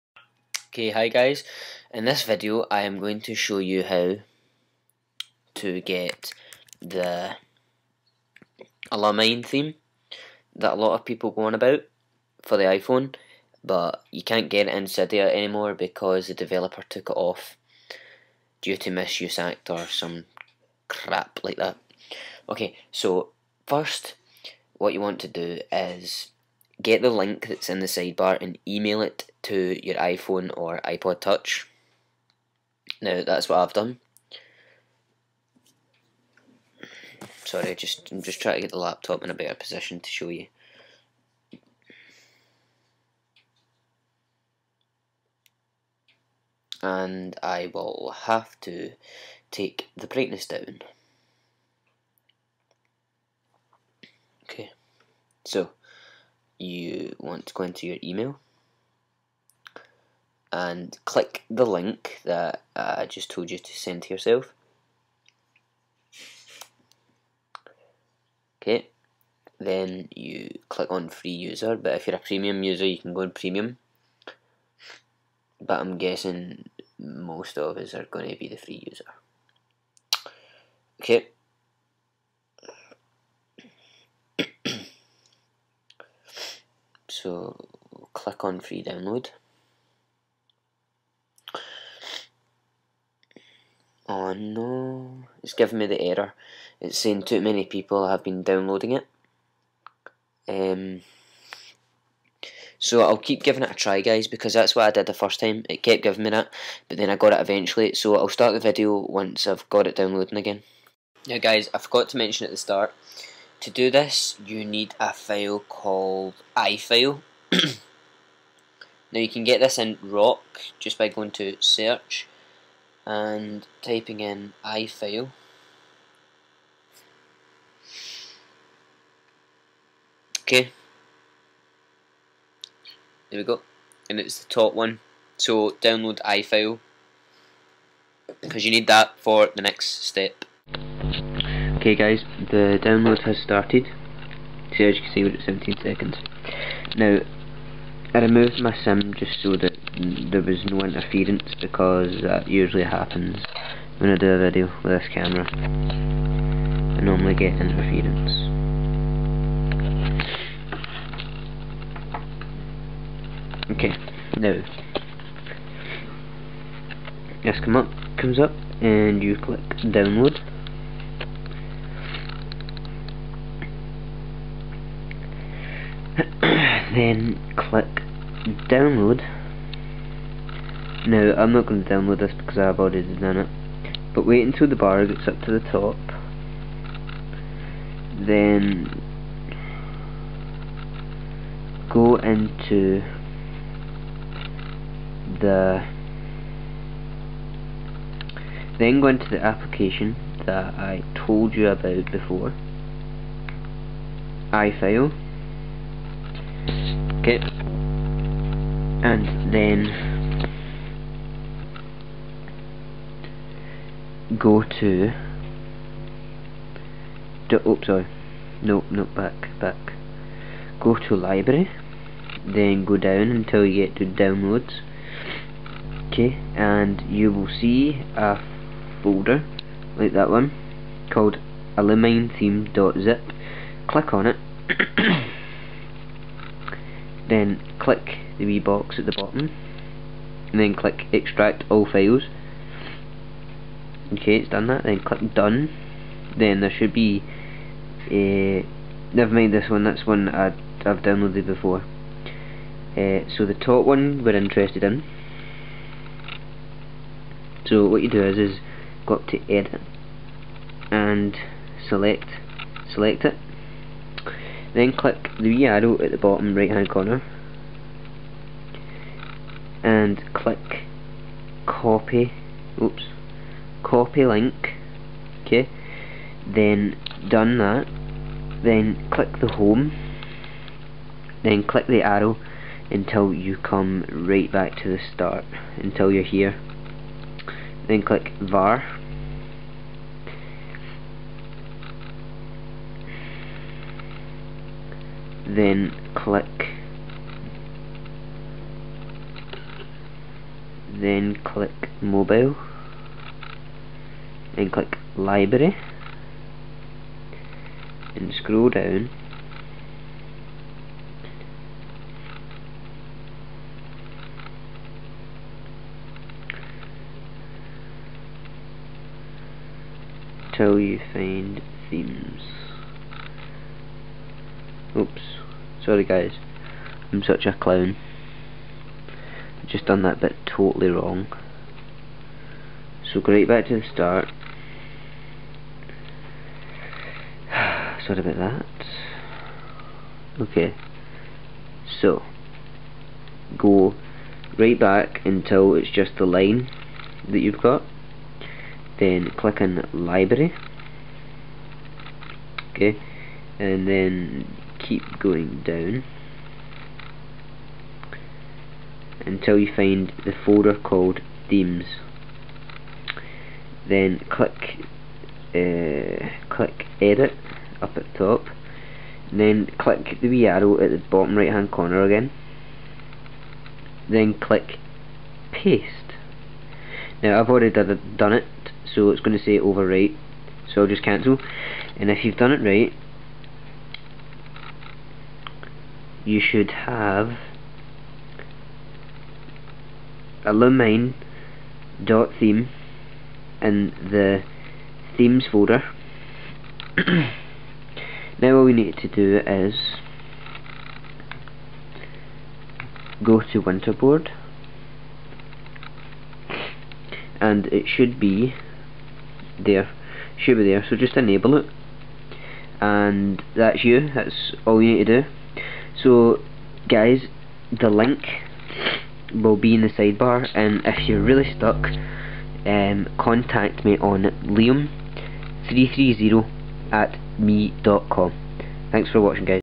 how to get illumine iPhone Theme
This is a tutorial of how to get the illumine theme for iPhone/iPod Touch because it is no longer in cydia.

Also you will need to install installous!